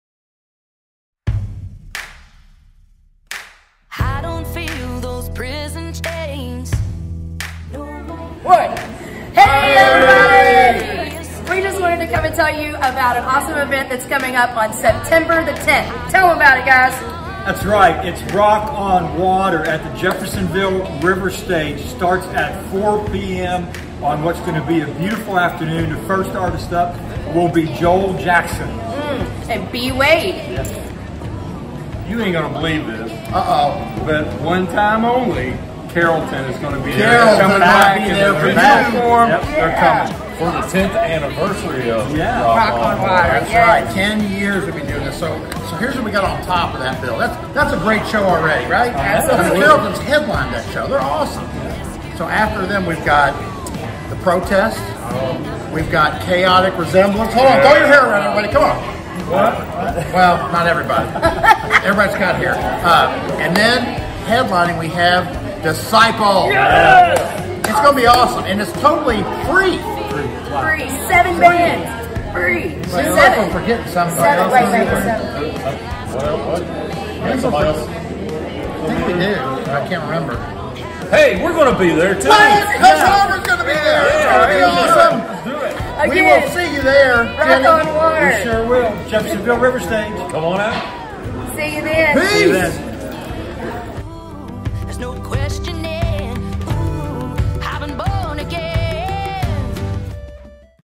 Hush Harbor at ROCK ON WATER FESTIVAL 9 -10-22 at 4PM
Hush Harbor will be a part of Rock On Water on 9-10-22 starting at 4PM along with Joel Jackson, B Wade, CARROLLTON, Chaotic Resemblance, The Protest and DISCIPLE. All Free thanks to Kaiser Home Support Services and all of the other sponsors! AT THE JEFFERSONVILLE RIVERSTAGE in JEFFERSONVILLE, INDIANA